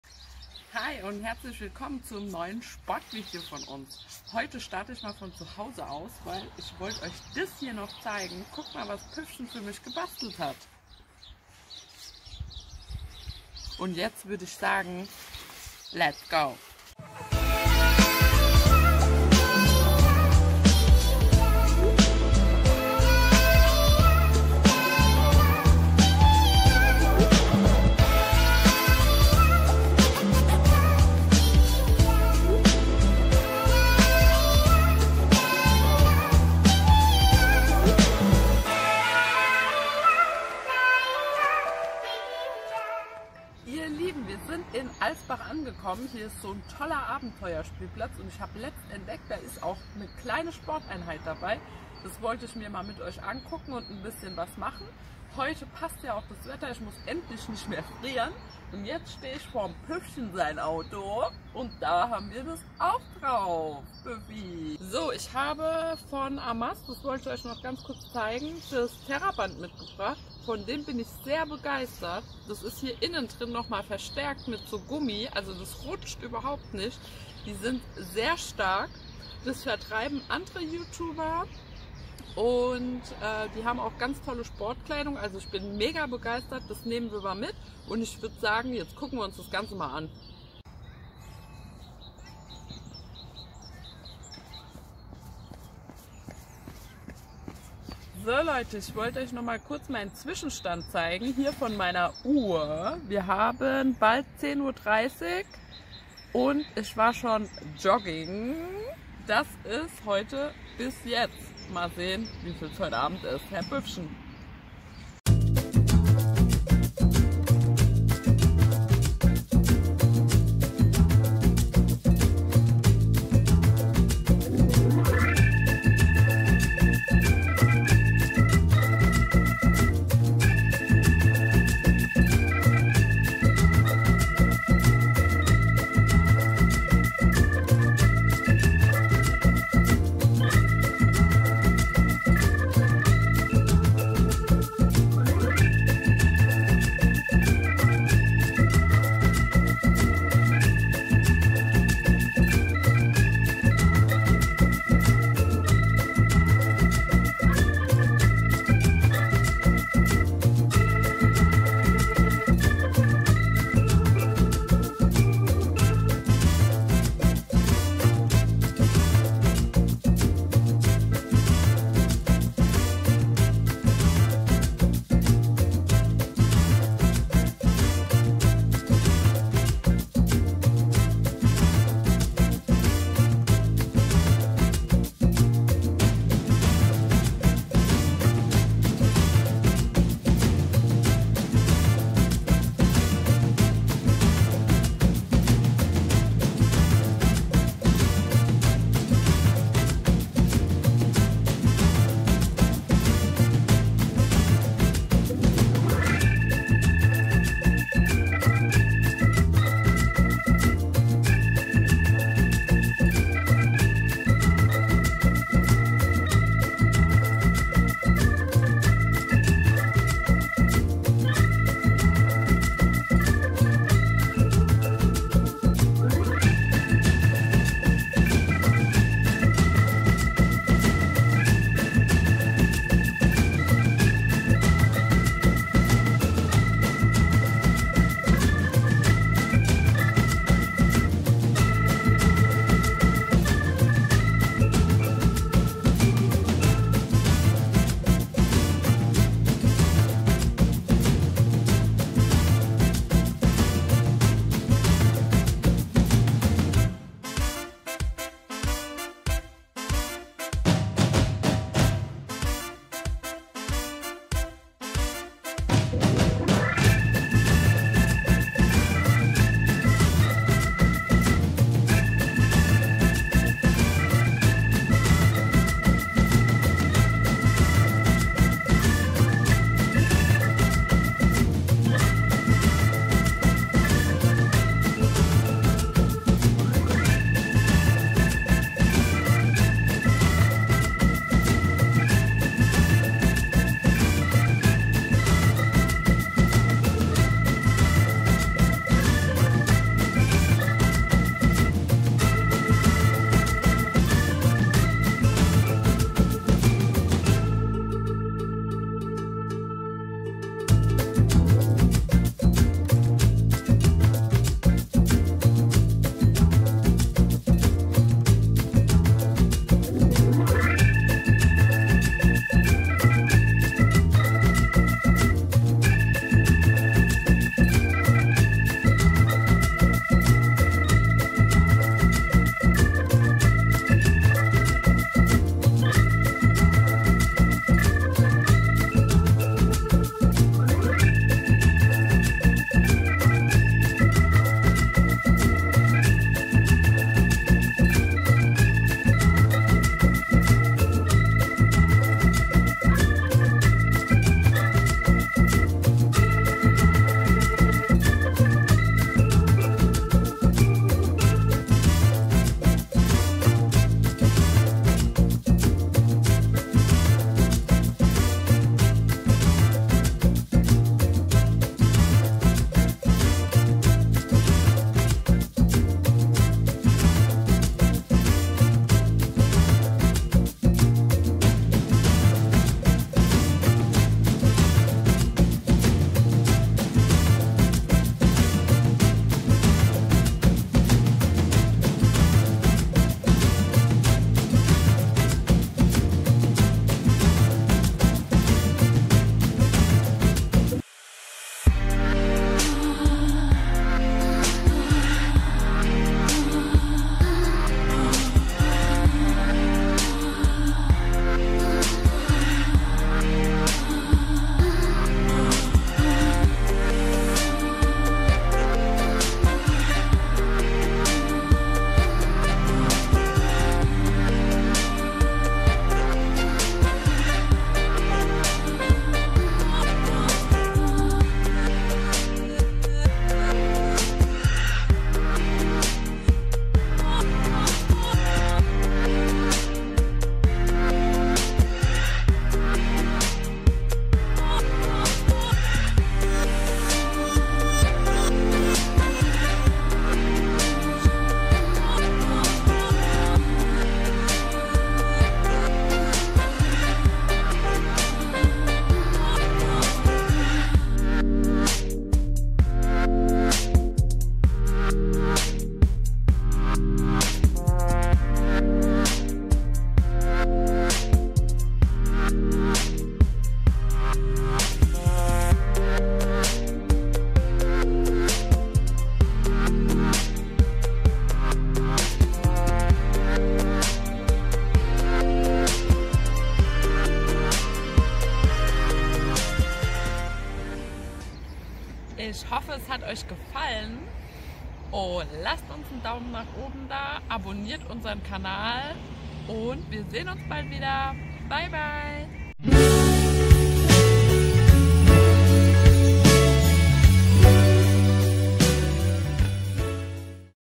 SPORT IM FREIEN | am liebsten im Natur Gym-Studio 🌳🌲 | die PÜFFCHENS
Heute geht es mal wieder an die frische Luft zum Workout...

Ich zeige euch was man alles so machen kann..

Und hoffe natürlich euch gefällt es